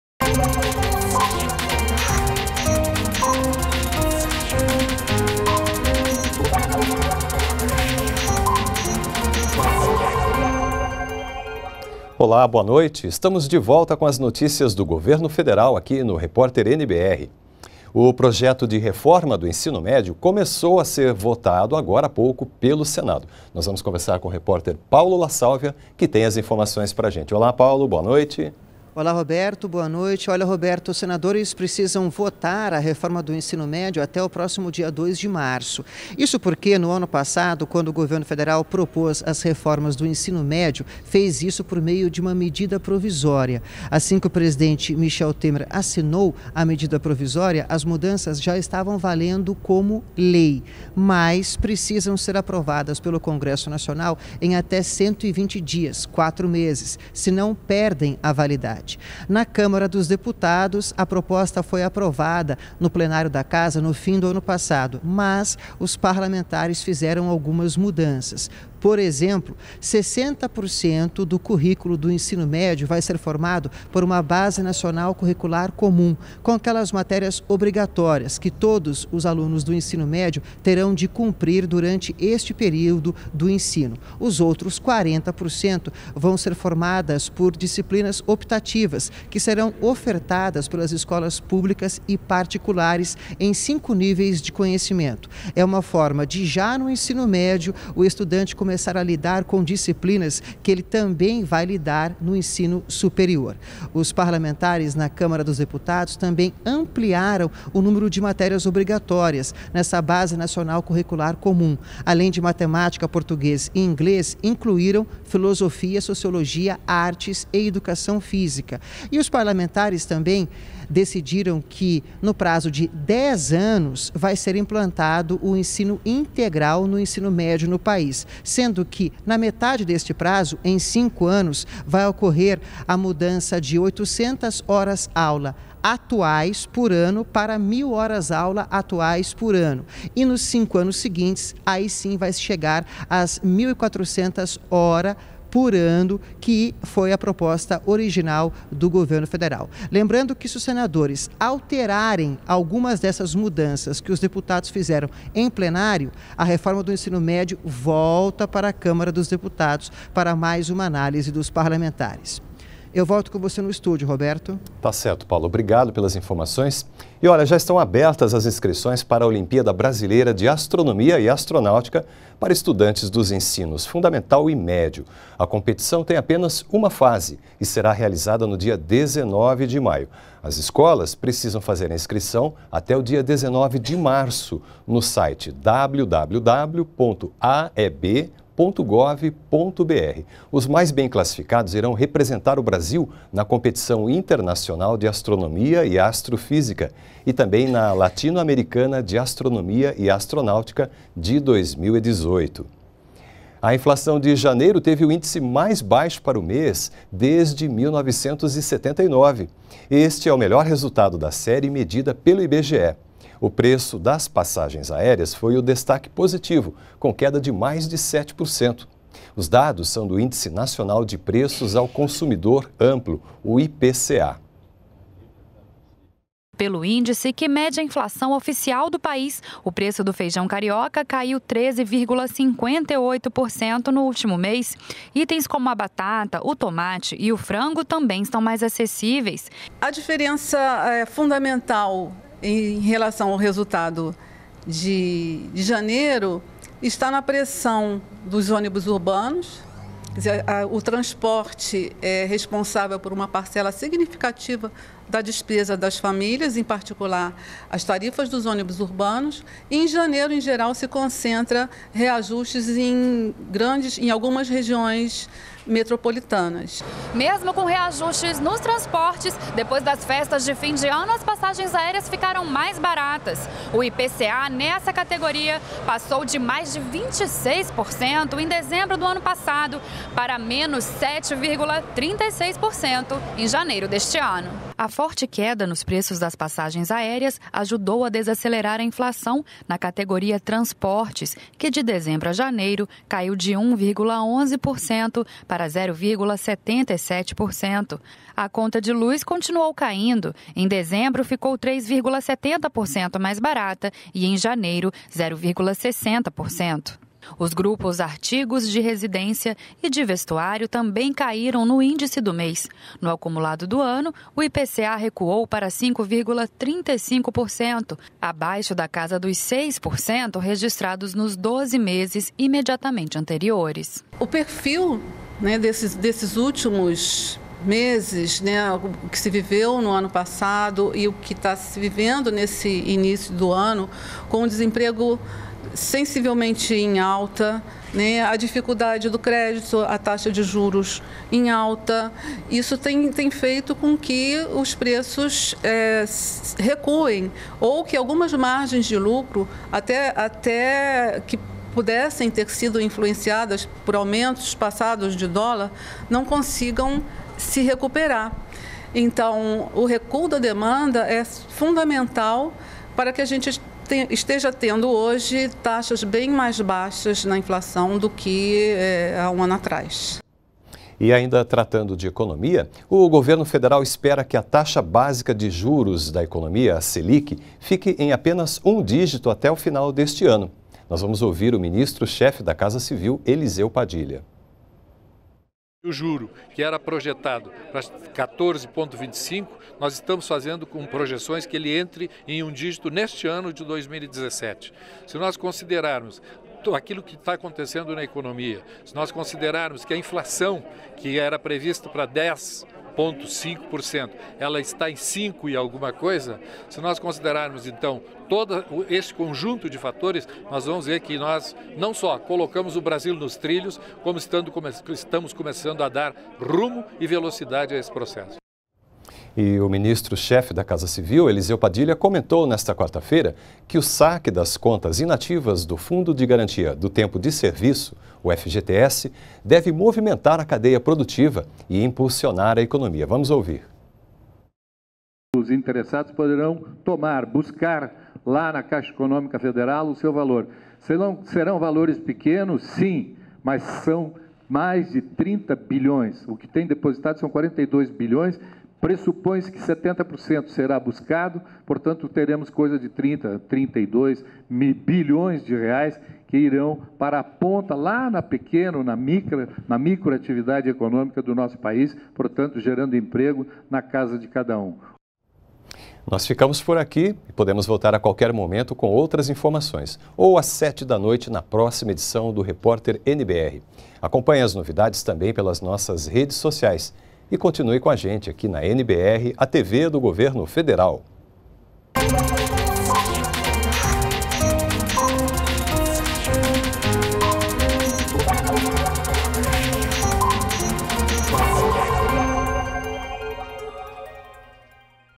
Projeto de reforma do ensino médio começa a ser votado pelo Senado
REPÓRTER NBR 18H - 08.02.17: Projeto de reforma do ensino médio começou a ser votado pelo Senado. E mais: já estão abertas as inscrições para a Olimpíada Brasileira de Astronomia e Astronáutica para estudantes dos ensinos fundamental e médio. A inflação de janeiro teve o índice mais baixo para o mês desde 1979.